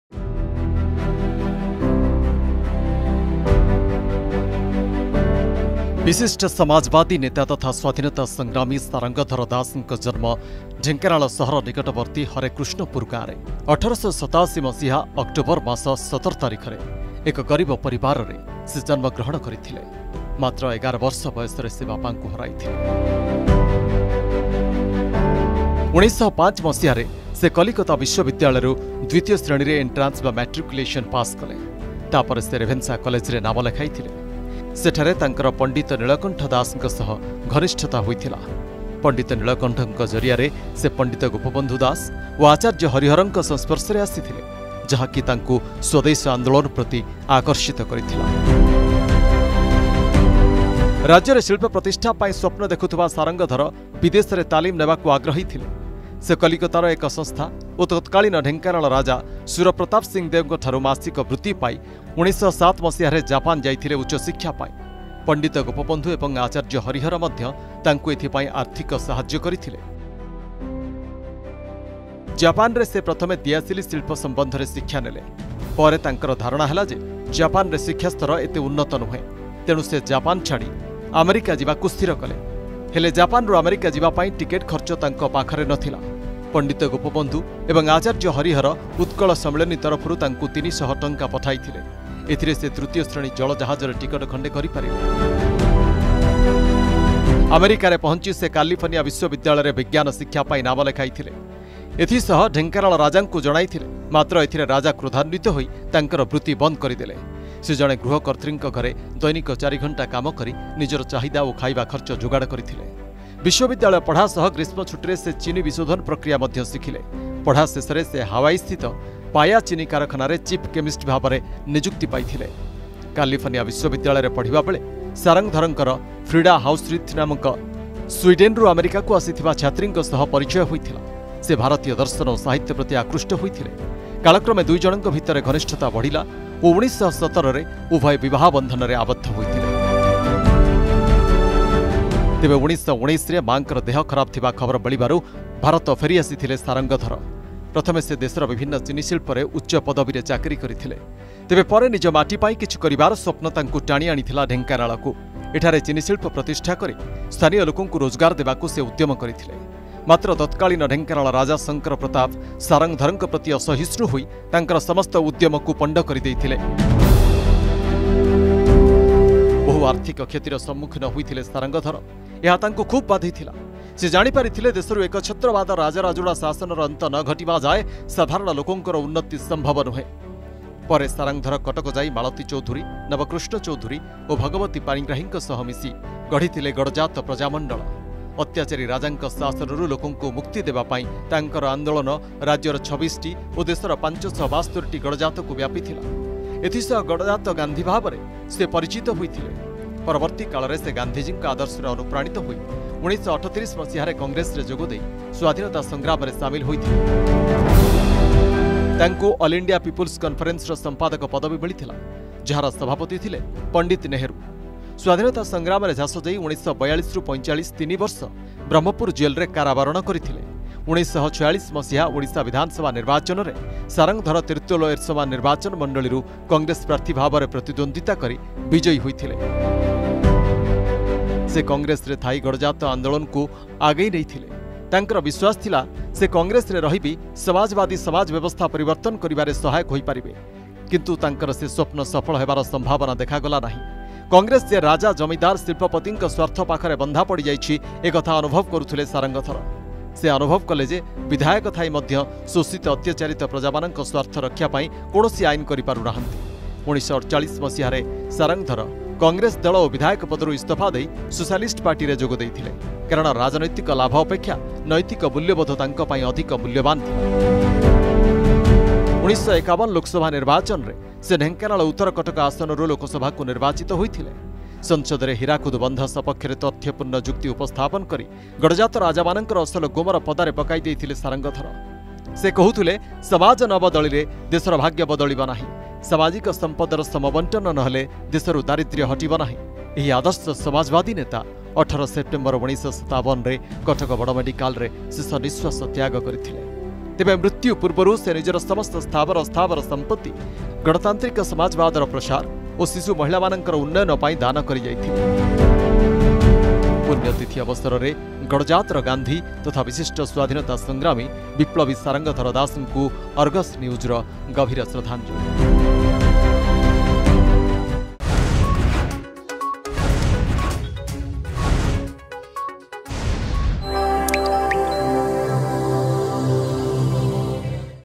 ନିସ୍ବାର୍ଥପର ସେବା ଯୋଗୁଁ ସେ ଗଡଜାତର ଗାନ୍ଧୀ ରୂପରେ ପରିଚିତ ଥିଲେ
ବିଶିଷ୍ଟ ସମାଜବାଦୀ ନେତା ତଥା ସ୍ବାଧୀନତା ସଂଗ୍ରାମୀ ସାରଙ୍ଗଧର ଦାସଙ୍କ ଜନ୍ମ ଢେଙ୍କାନାଳ ସହର ନିକଟବର୍ତ୍ତୀ ହରେକୃଷ୍ଣପୁର ଗାଁରେ । ସେ ଥିଲେ ଓଡିଶାରେ ସମାଜବାଦୀ ଆନ୍ଦୋଳନର କଣ୍ଣଧାର ।
ଥିଲେ ମଧ୍ୟ ଜଣେ ସାଲିସ ବିହୀନ ରାଜନେତା